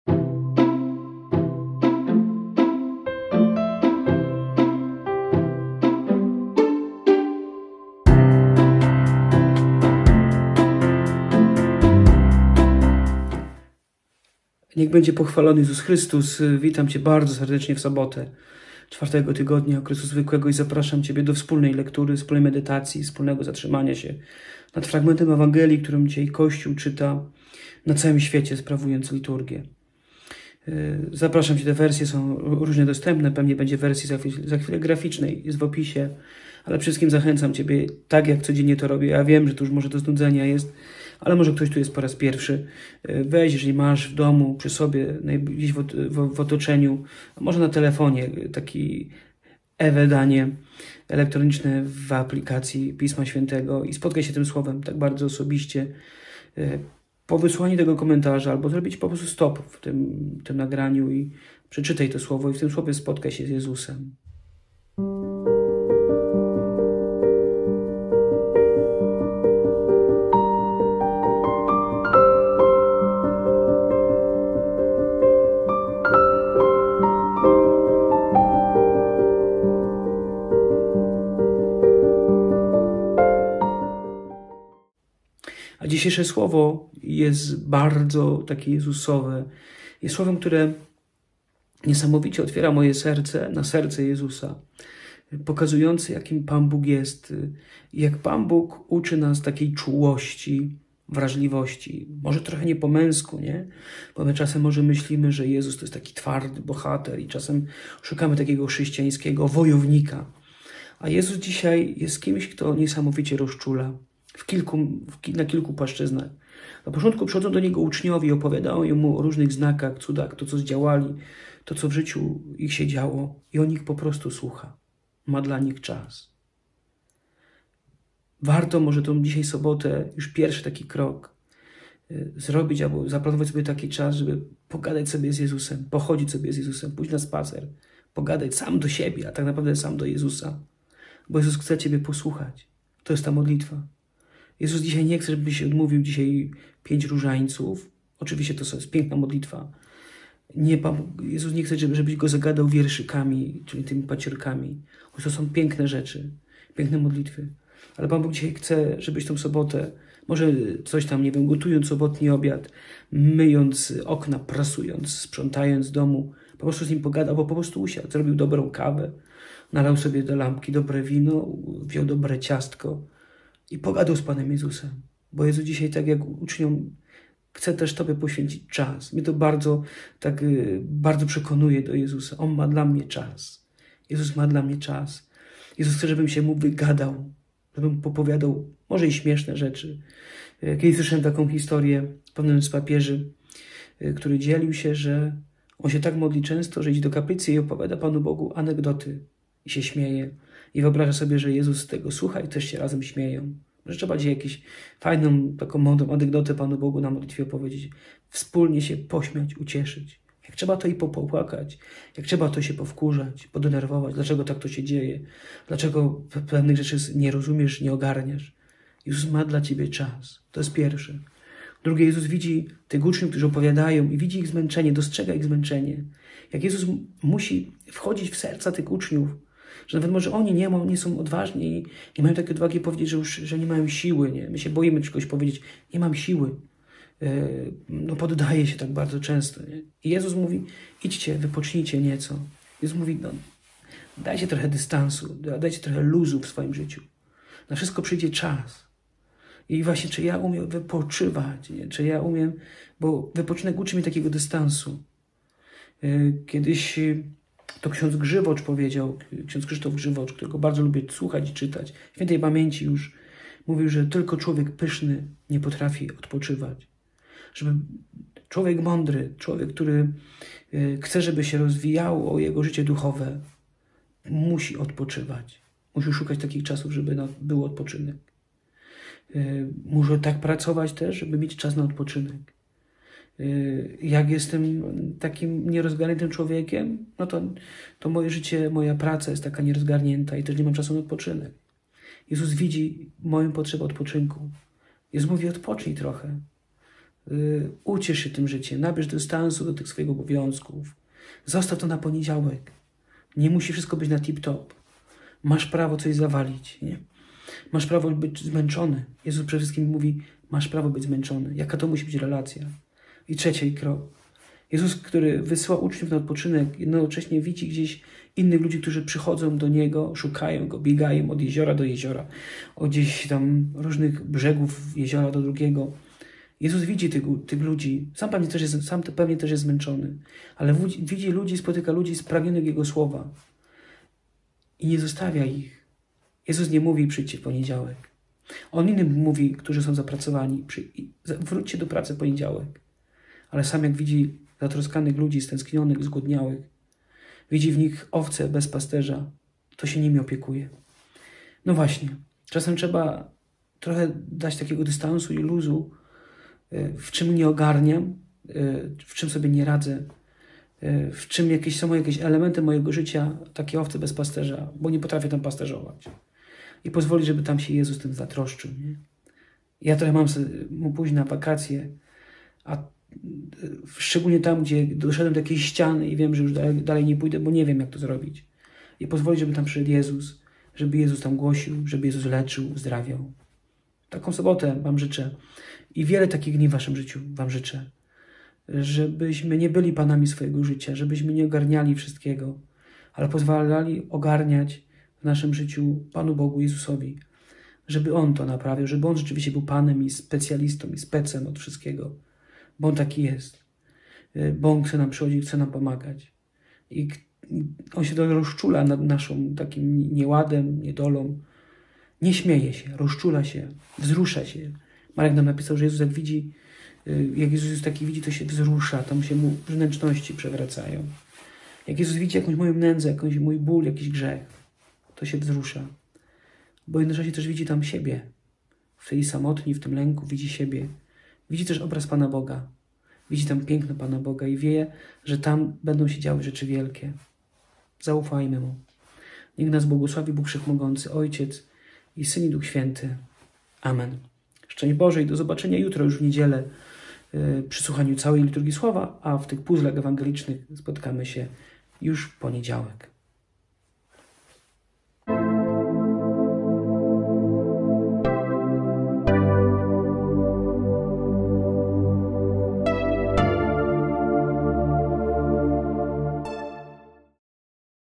Puzzle Ewangelii |24| odpocznij-wyluzuj
Apostołowie zebrali się u Jezusa i opowiedzieli Mu wszystko, co zdziałali i czego nauczali. A On rzekł do nich: «Pójdźcie wy sami osobno na pustkowie i wypocznijcie nieco». Tak wielu bowiem przychodziło i odchodziło, że nawet na posiłek nie mieli czasu.
Odpłynęli więc łodzią na pustkowie, osobno. Lecz widziano ich odpływających. Wielu zauważyło to i zbiegli się tam pieszo ze wszystkich miast, a nawet ich wyprzedzili.
Gdy Jezus wysiadł, ujrzał wielki tłum. Zlitował się nad nimi, byli bowiem jak owce niemające pasterza. I zaczął ich nauczać o wielu sprawach.